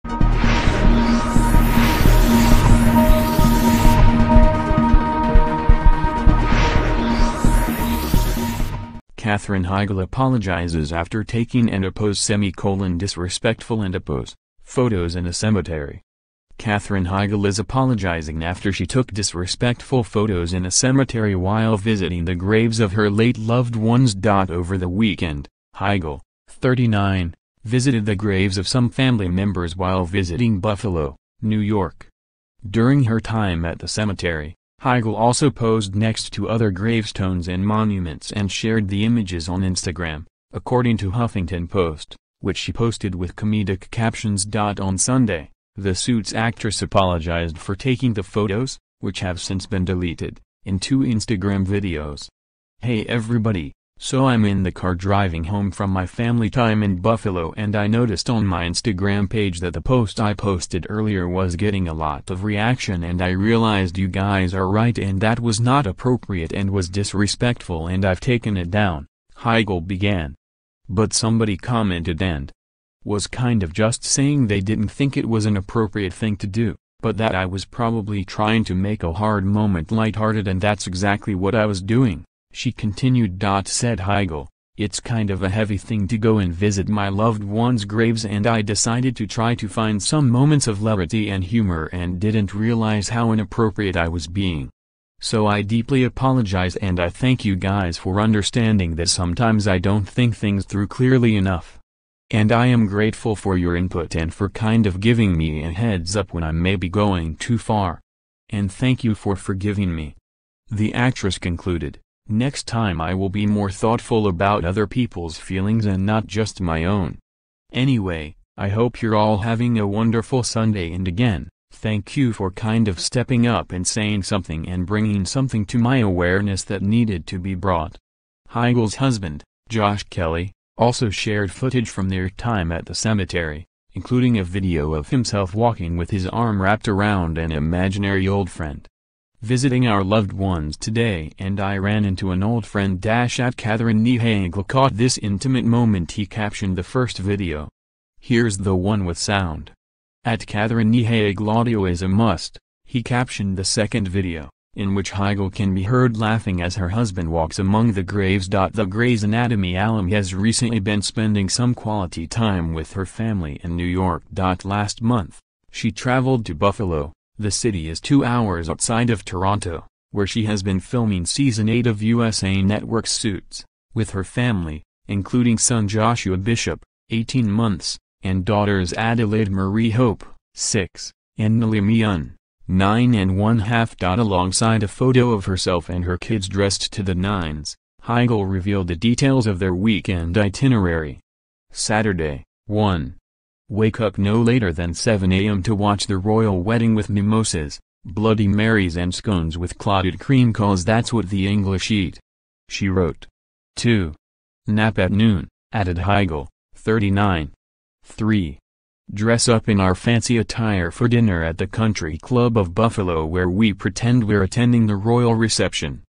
Katherine Heigl Apologizes After Taking &apos;Disrespectful&apos; Photos in a Cemetery
Katherine Heigl Apologizes After Taking &apos;Disrespectful&apos; Photos in a Cemetery. Katherine Heigl is apologizing after she took “disrespectful” photos in a cemetery while visiting the graves of her late loved ones.Over the weekend, Heigl, 39, visited the graves of some family members while visiting Buffalo, New York. During her time at the cemetery, Heigl also posed next to other gravestones and monuments and shared the images on Instagram, according to Huffington Post, which